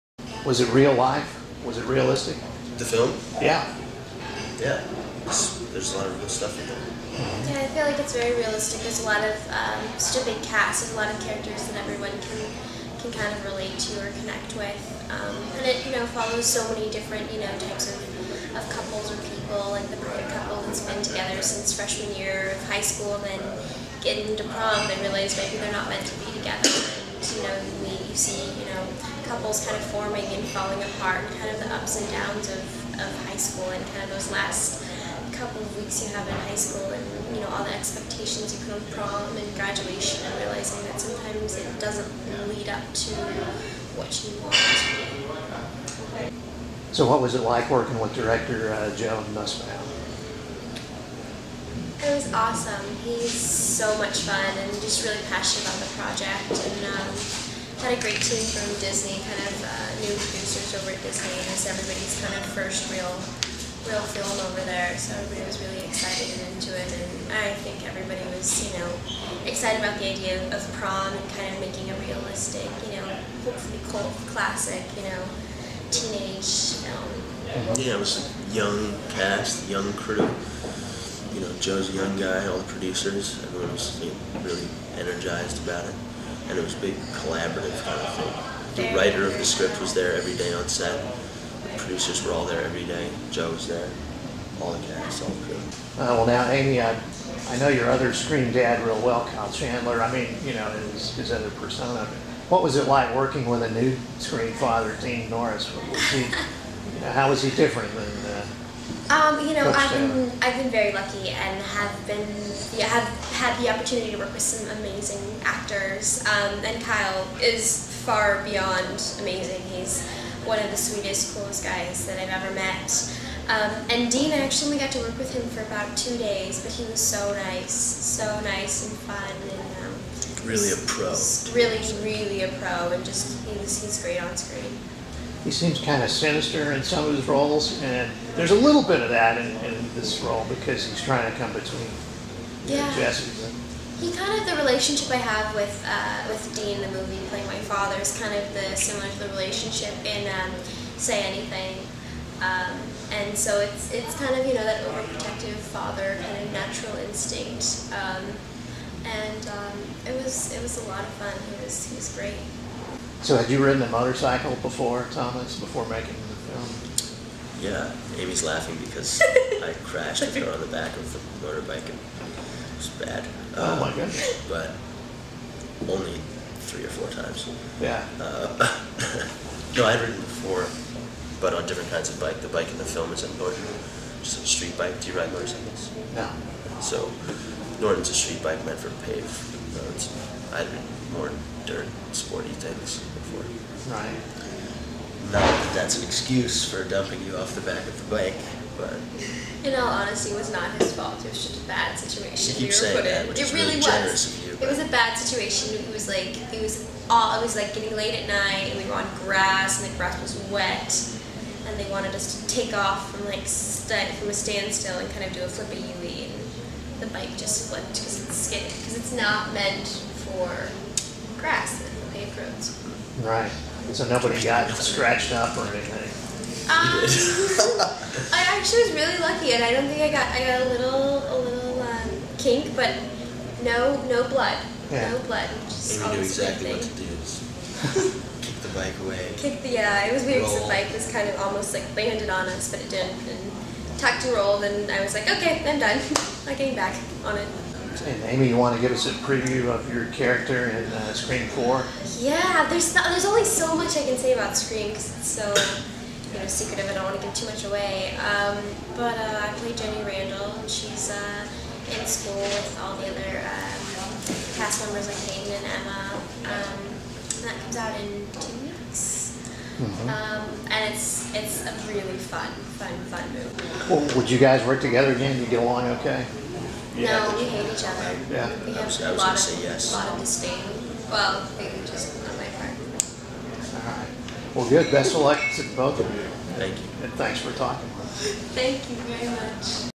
Video interview: Aimee Teegarden and Thomas McDonell talk about Prom
Aimee Teegarden and Thomas McDonell were in Dallas last Wednesday, on a tour to promote their new film Prom, which opens nationwide on April 29.
Being a big fan of Aimee's role as coach's daughter Julie Taylor on Friday Night Lights, I was pleased to have a chance to speak with her and Thomas about their work in the PG-rated Disney... Read the full story

Here's the review for Prom from John P. Meyer, film guy,